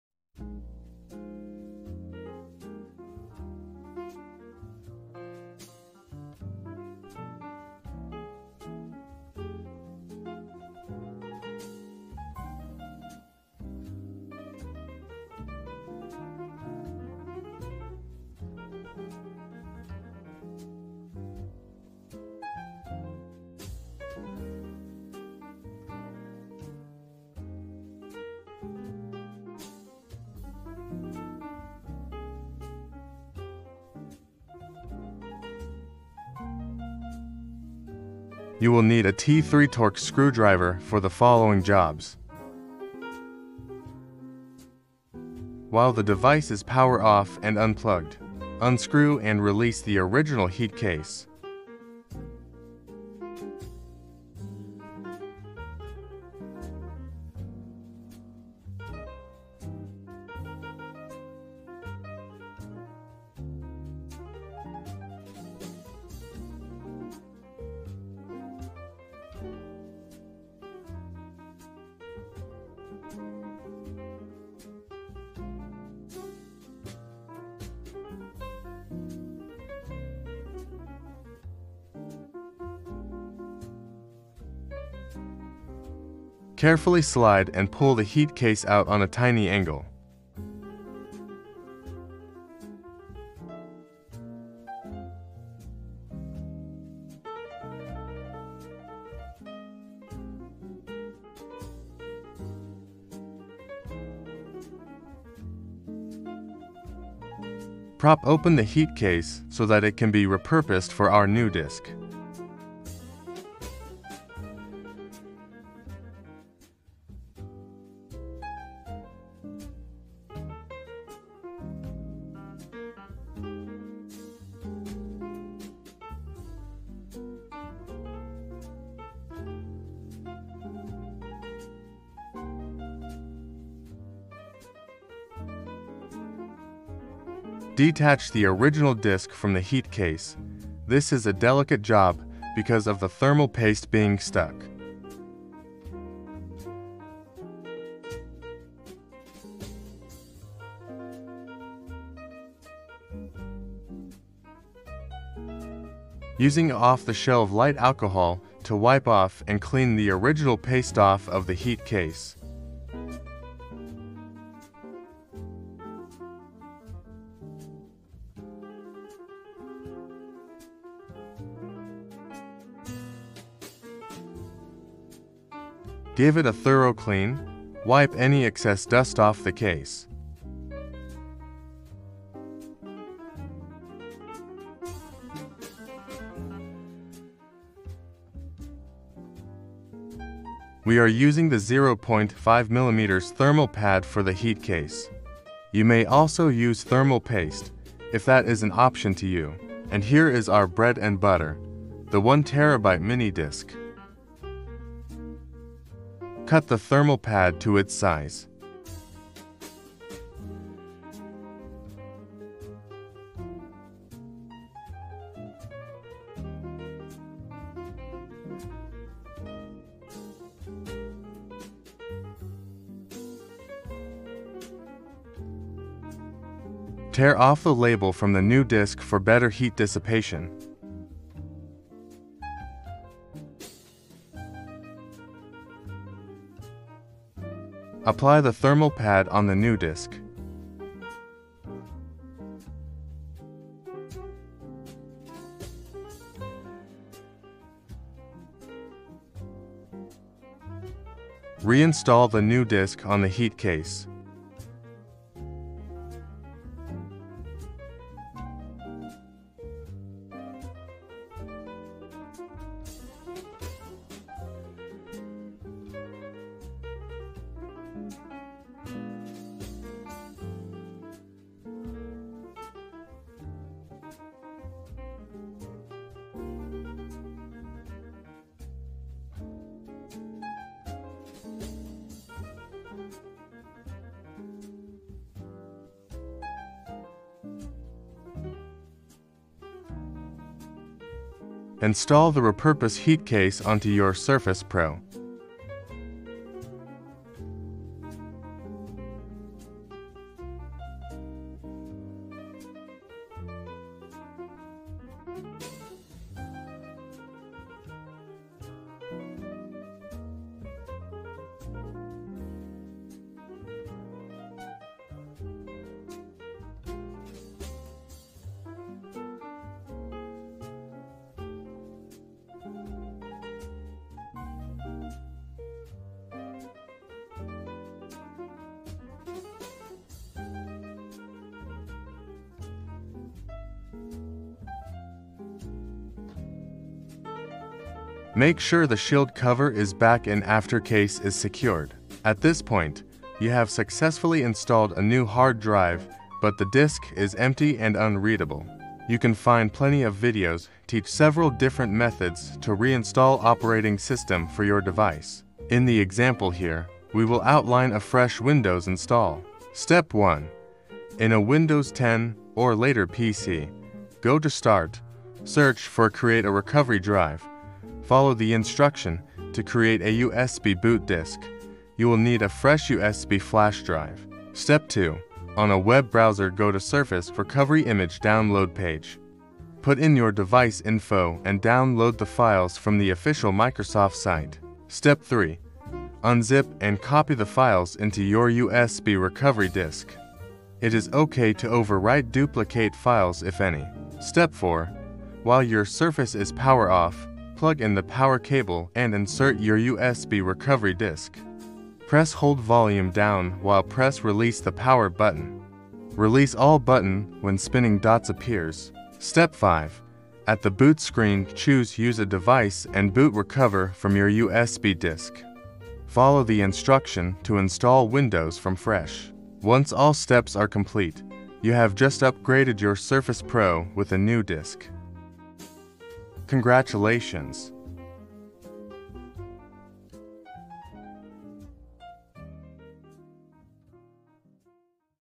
[DIY Series]💻Microsoft Surface Pro 9💾1TB SSD Swap (with original Heat Case)
Things you would likely need to upgrade a Surface Pro's SSD hard drive:

SURFACE PRO 9 x1
Mini SSD (M.2 NVMe) x1
Any USB flash drive x1
USB-C flash drive adaptor (optional) x1
Thermal Pad (or paste) x1
T3 Torx Screwdriver x1

A set of delicate hand is highly recommended.

Reason for doing this is to give you better performance, more choices, all at a lower cost (instead of buying a straight spec-out OEM).